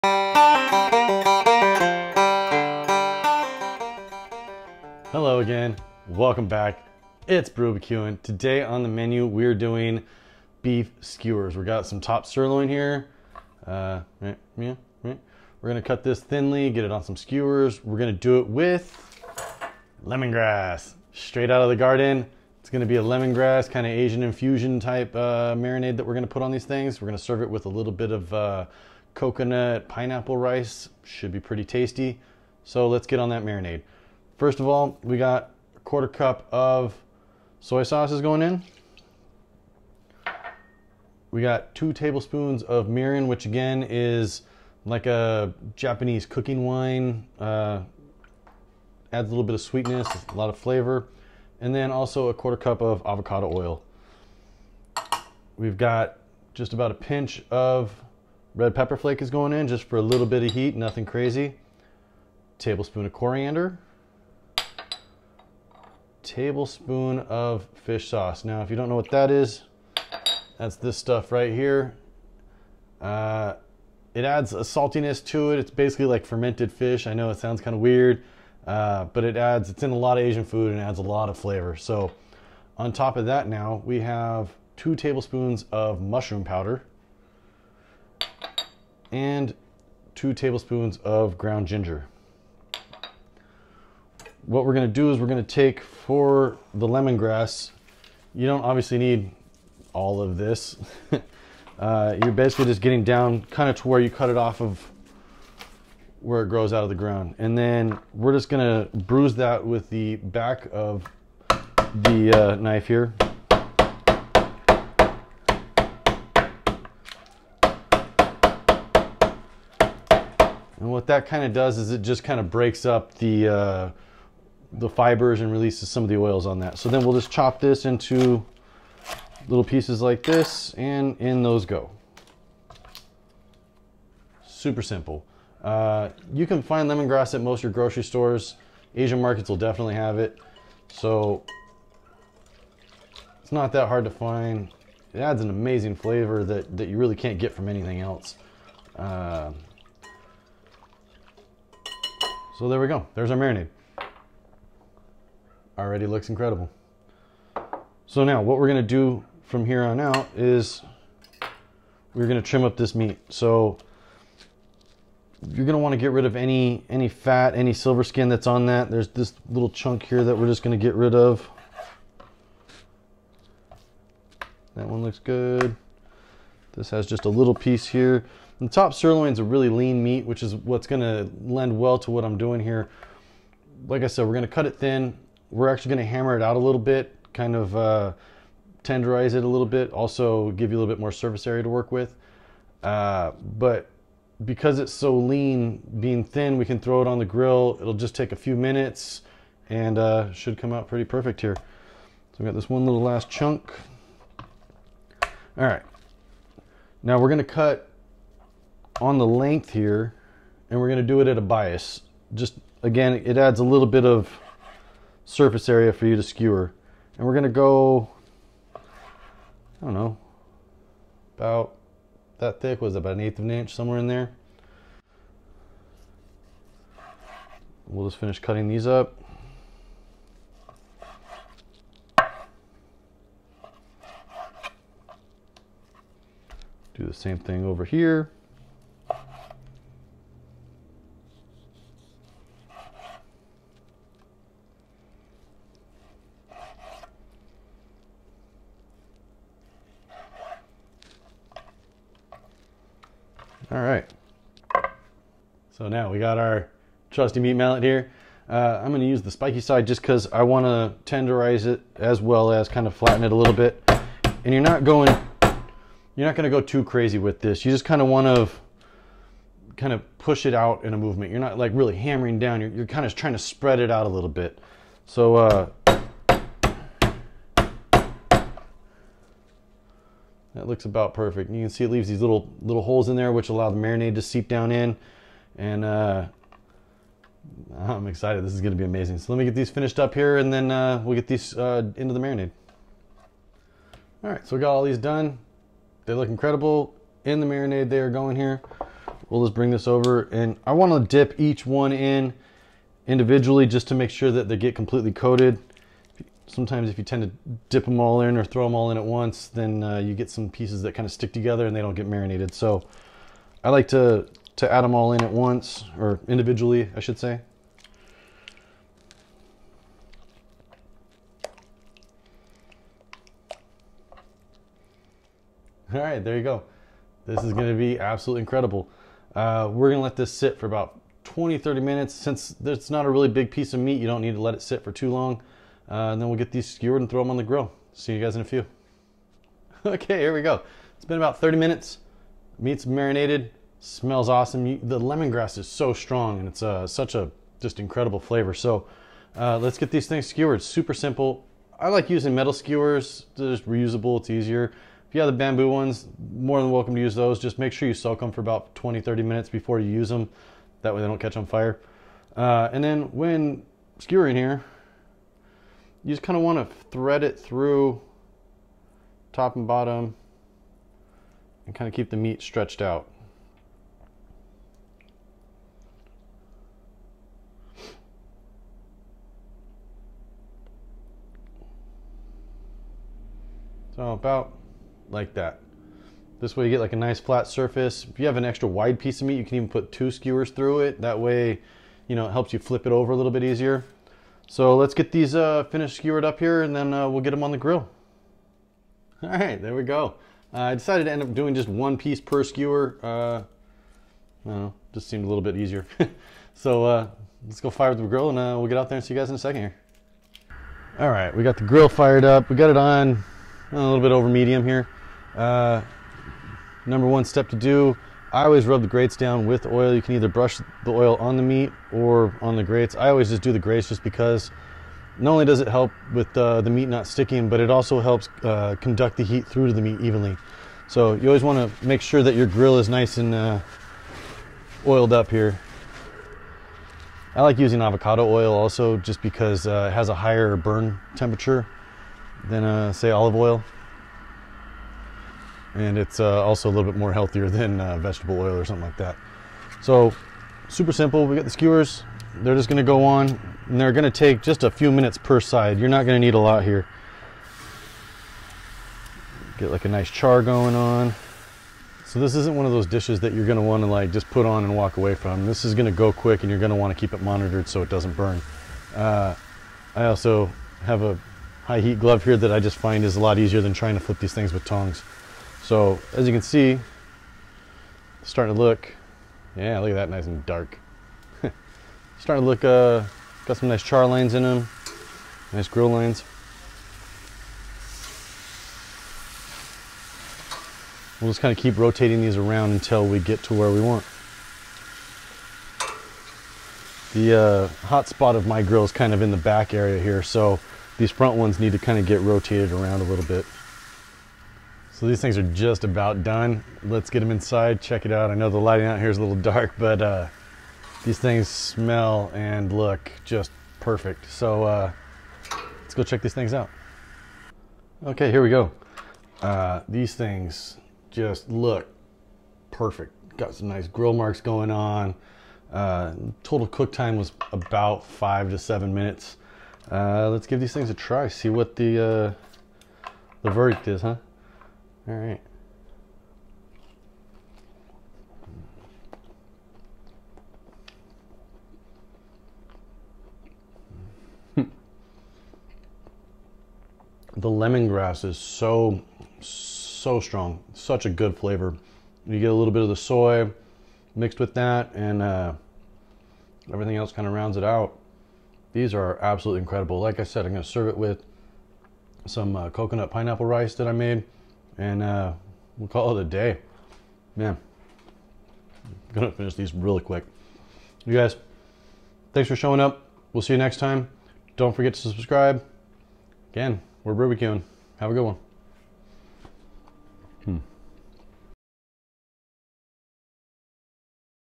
How to Make Top Sirloin Lemongrass Skewers / BrewBQ'n
In this video I will be making beef skewers out of top sirloin in an Asian inspired marinade.  They are super simple and incredibly good.  I tenderize the top sirloin with a meat mallet to not only flatten the meat but to also create crevasses for the marinade to soak into. The recipe for the marinade will be listed below.
Marinade
1/4 cup soy sauce
2 tbsp mirin
1/4 cup avocado oil
dash red chili flakes
1 tbsp coriander
2 tbsp fish sauce
2 tbsp mushroom powder
2 tbsp ground ginger